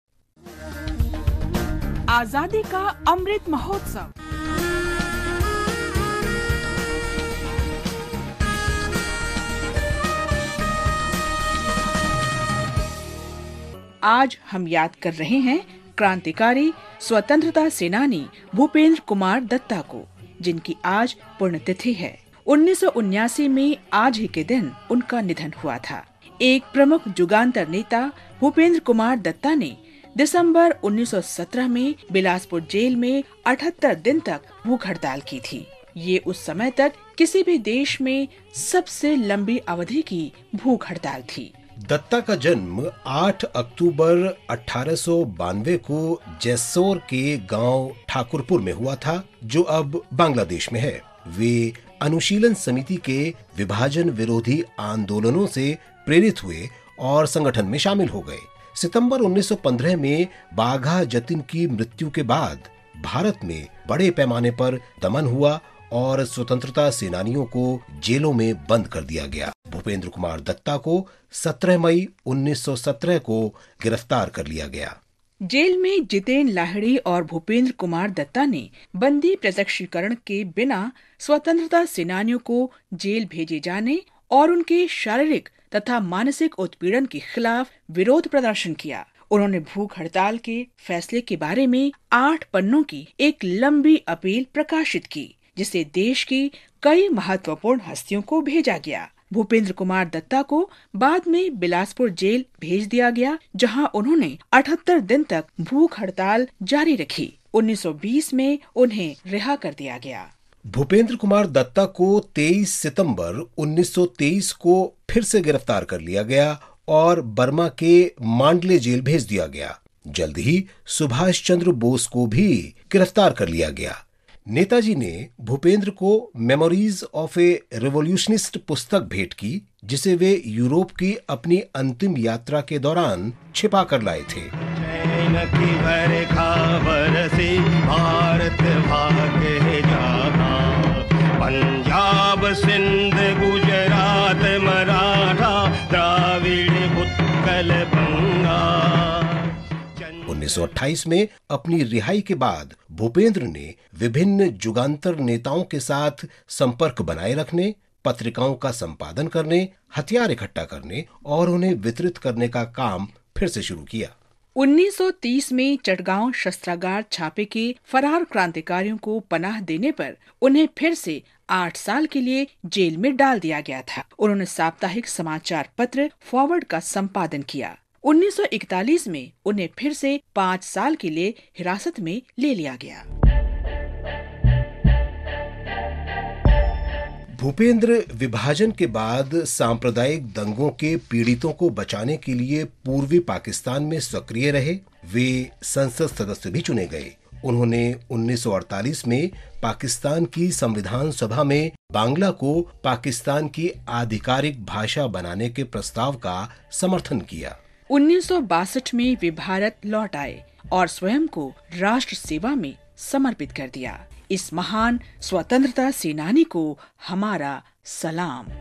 क्रांतिकारी स्वतंत्रता सेनानी भूपेंद्र कुमार दत्ता
29 दिसंबर, क्रांतिकारी स्वतंत्रता सेनानी भूपेंद्र कुमार दत्ता की पुण्यतिथि है। 29 दिसंबर, 1979 को आज ही के दिन उनका निधन हुआ था। एक प्रमुख जुगांतर नेता, भूपेंद्र कुमार दत्ता ने दिसंबर 1917 में बिलासपुर जेल में 78 दिनों के लिए भूख हड़ताल की थी । यह उस समय तक किसी भी देश में सबसे लंबी अवधि की भूख हड़ताल थी।
दत्ता का जन्म 8 अक्टूबर, 1892 को जेस्सोर के गांव ठाकुरपुर में हुआ था जो अब बांग्लादेश में है। वे अनुशीलन समिति के विभाजन विरोधी आंदोलनों से प्रेरित हुए और संगठन में शामिल हो गए। सितंबर 1915 में, बाघा जतिन की मृत्यु के बाद, भारत में बड़े पैमाने पर दमन हुआ और स्वतंत्रता सेनानियों को जेलों में बंद कर दिया गया। भूपेंद्र कुमार दत्ता को 17 मई 1917 को गिरफ्तार कर लिया गया । जेल में, जितेन लाहिड़ी और भूपेंद्र कुमार दत्ता ने बंदी प्रत्यक्षीकरण के बिना स्वतंत्रता सेनानियों को जेल भेजे जाने और वहां उन पर शारीरिक तथा मानसिक अत्याचार के खिलाफ विरोध प्रदर्शन किया। उन्होंने भूख हड़ताल के फैसले के बारे में आठ पन्नों की एक लंबी अपील प्रकाशित की जिसे देश को सूचित करने के लिए कई महत्वपूर्ण हस्तियों को भेजा गया था। भूपेंद्र कुमार दत्ता को बाद में बिलासपुर जेल भेज दिया गया, जहां उन्होंने 78 दिनों तक भूख हड़ताल जारी रखी। उन्हें 1920 में रिहा किया गया था।